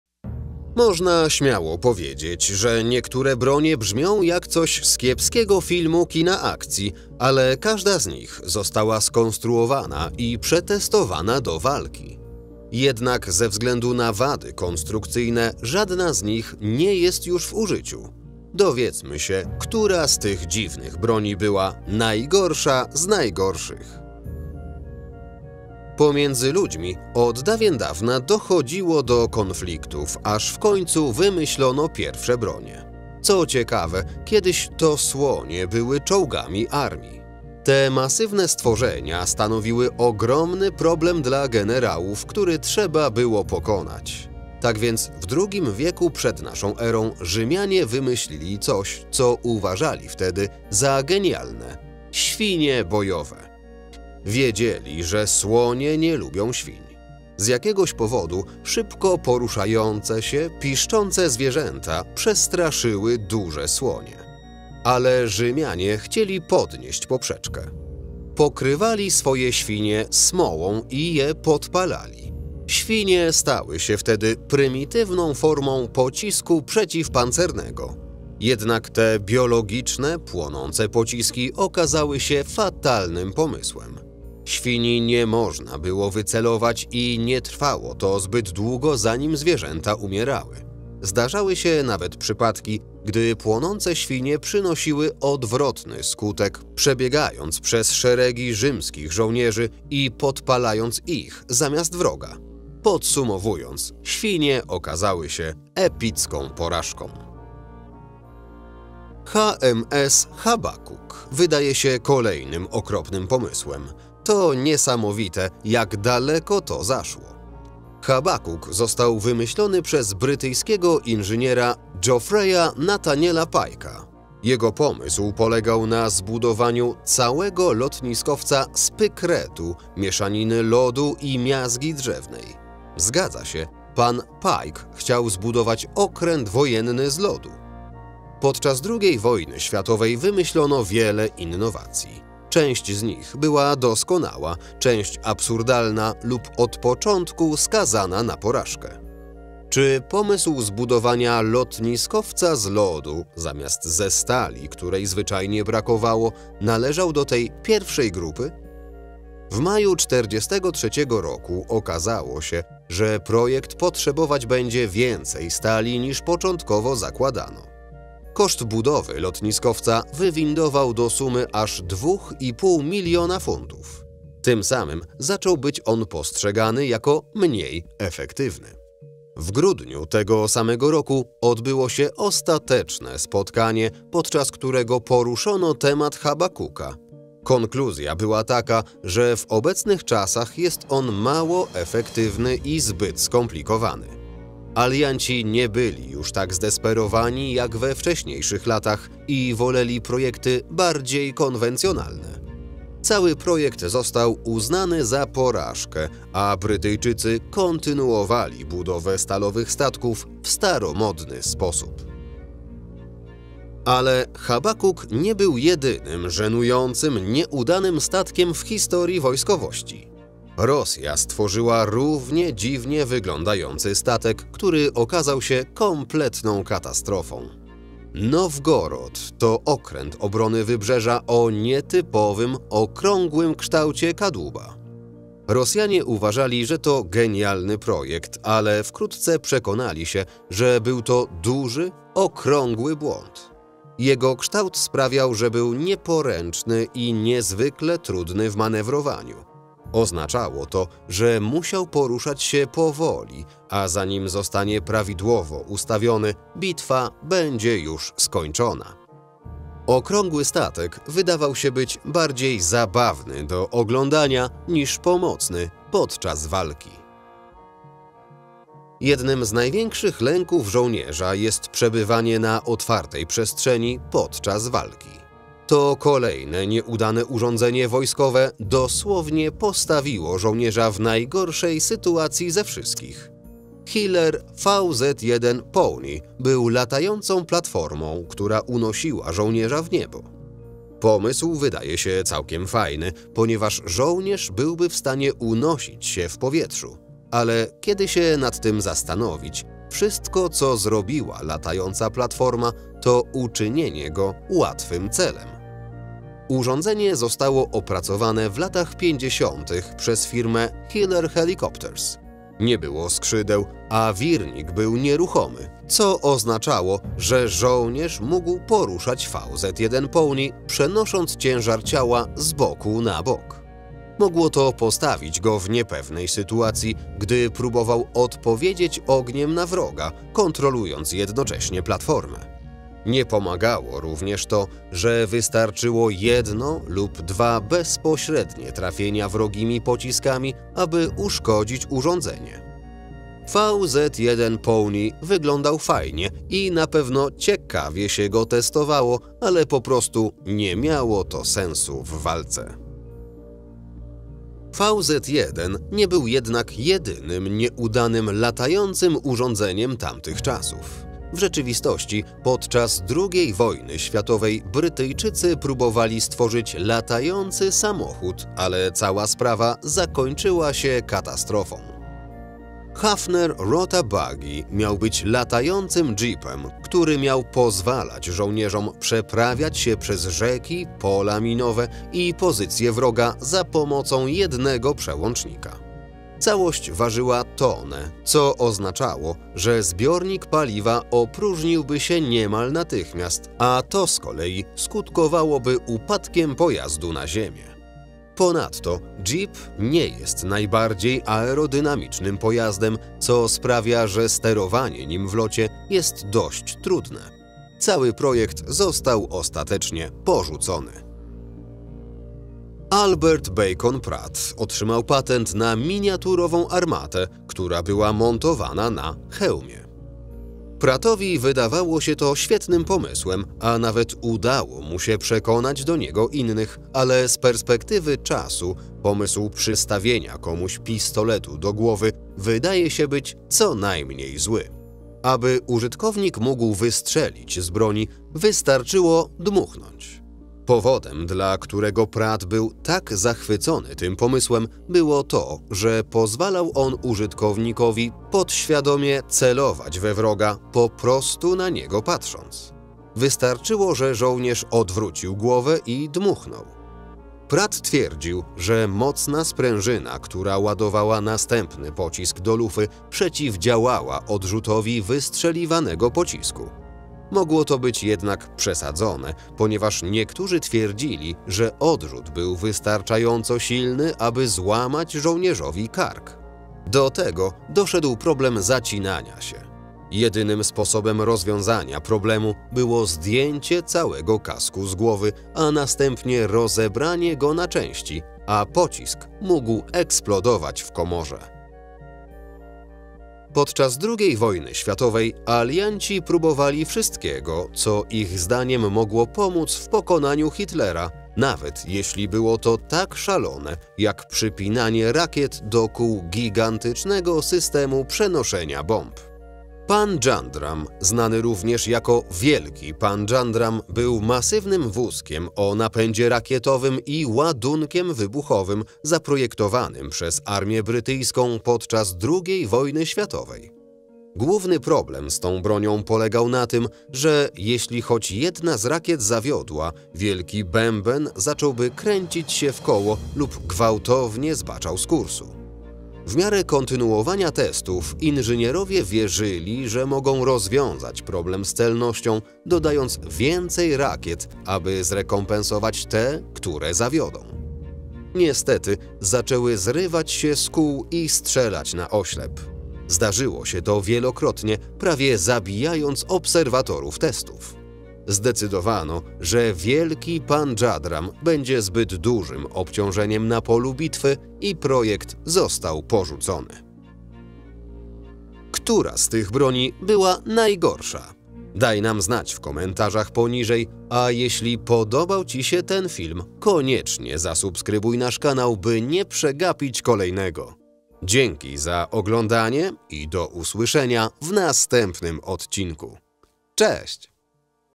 Najdziwniejsza broń używana w czasie II WŚ!
Można śmiało powiedzieć, że niektóre bronie brzmią jak coś z kiepskiego filmu kina akcji, ale każda z nich została skonstruowana i przetestowana do walki. Jednak ze względu na wady konstrukcyjne żadna z nich nie jest już w użyciu. Dowiedzmy się, która z tych dziwnych broni była najgorsza z najgorszych.